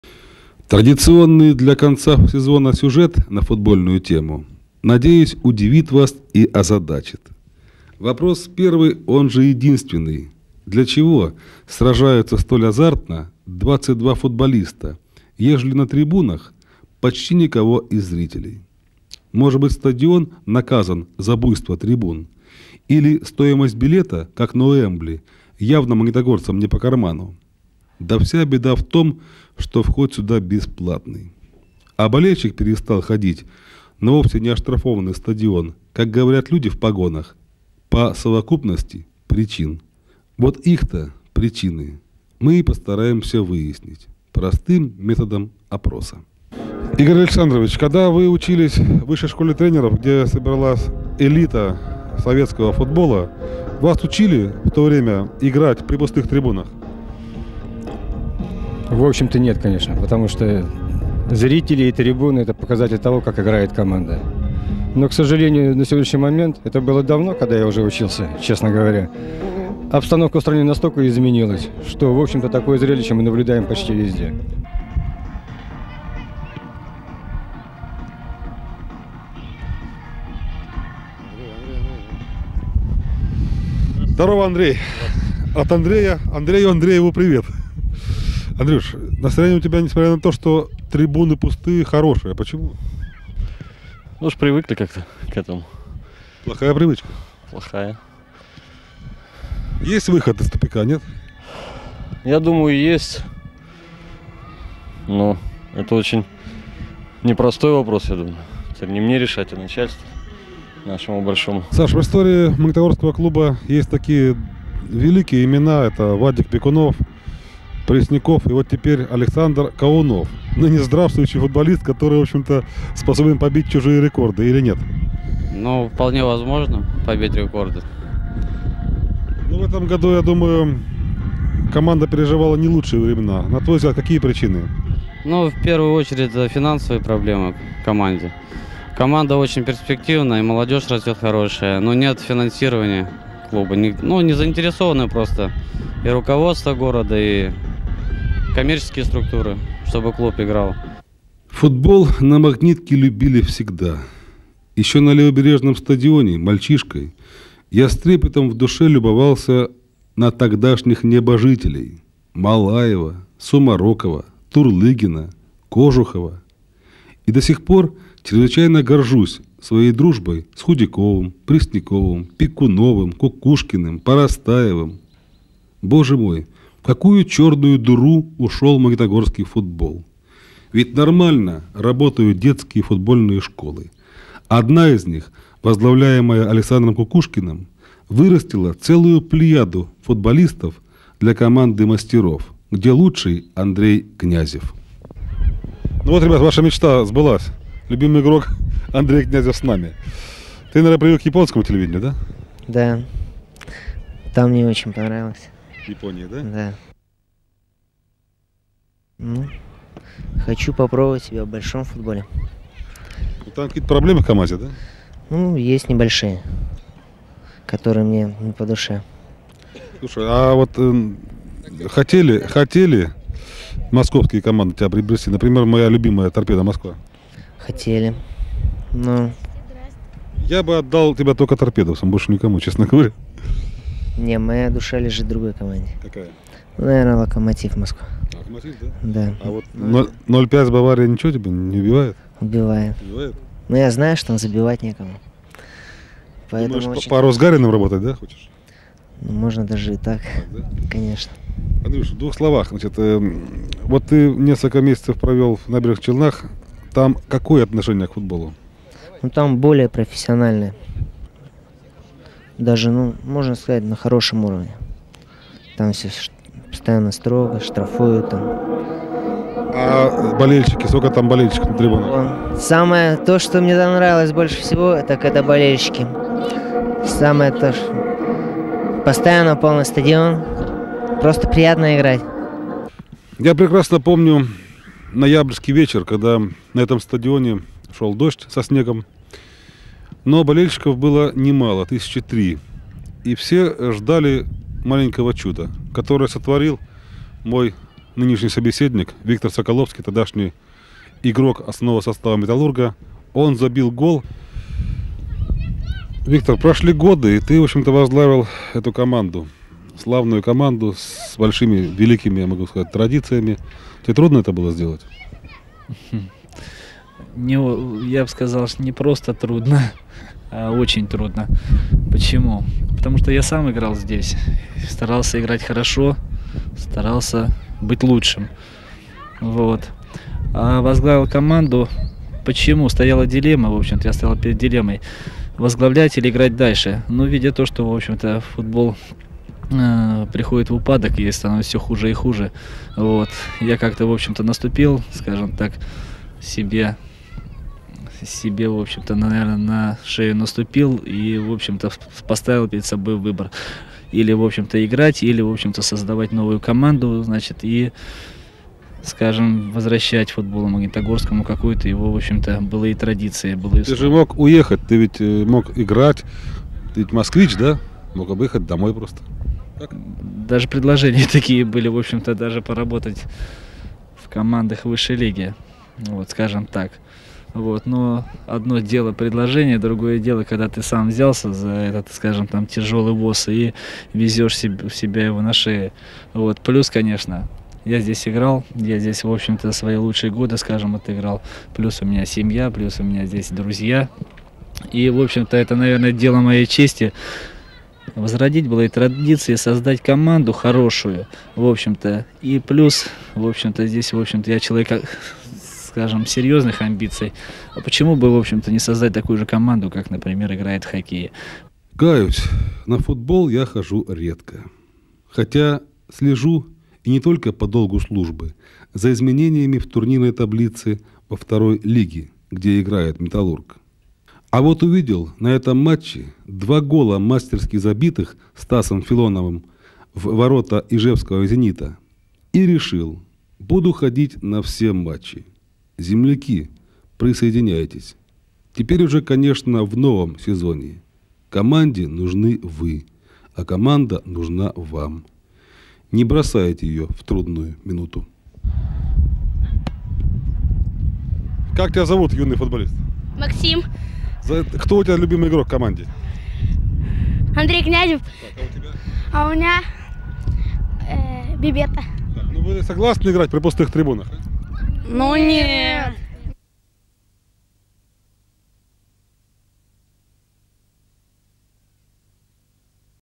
Football
Магнитогорский корреспондентский пункт ЧГТРК - проблемы футбола, Magnitogorsk correspondent offise of ChGTRK - the russian footboll problem in suburbia